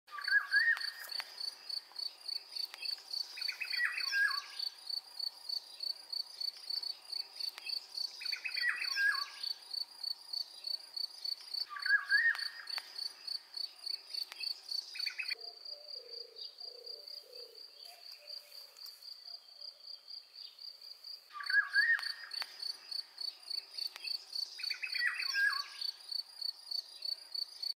Zorzal charlo y amapolas
El Zorzal Charlo (Turdus viscivorus), uno de los túrdidos que vive todo el año en los campos ibéricos, es corpulento con un vuelo directo, que es cuando se le observa la axila blanca que le distingue perfectamente de otros miembros de la familia